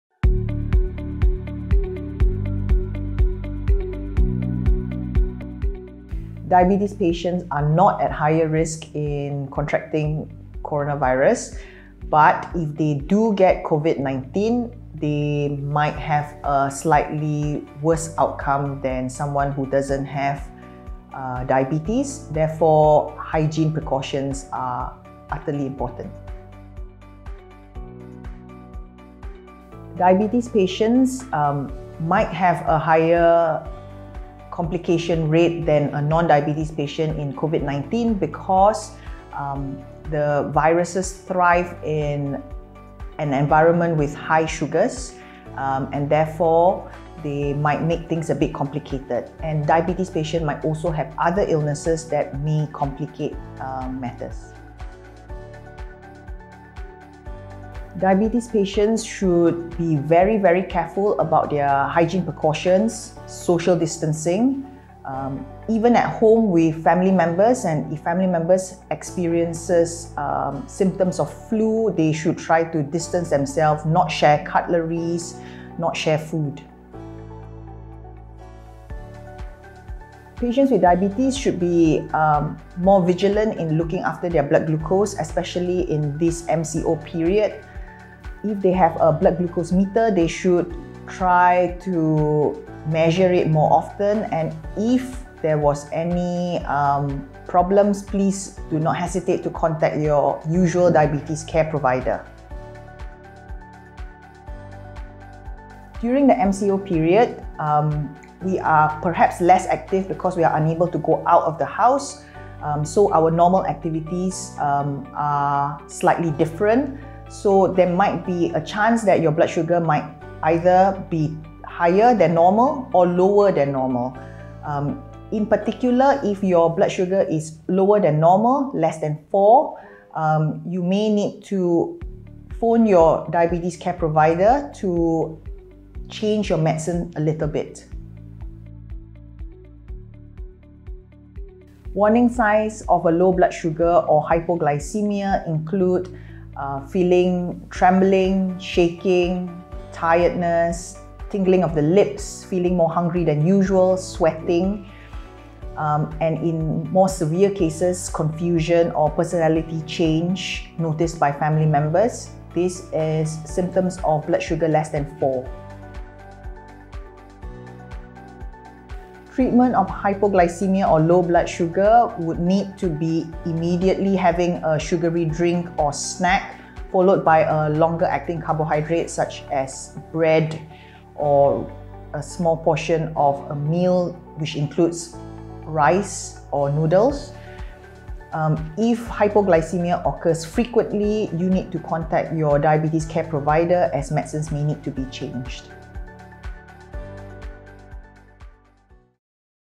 Ask SunMed Doctors: #2 Diabetes Care During COVID-19 by Dr Teoh Wei Leng
Covid-19 could potentially cause complications in diabetic patients, in this series, Dr Teoh Wei Leng, Consultant Physician and Endocrinologist, Sunway Medical Centre shares what these individuals need to pay attention to.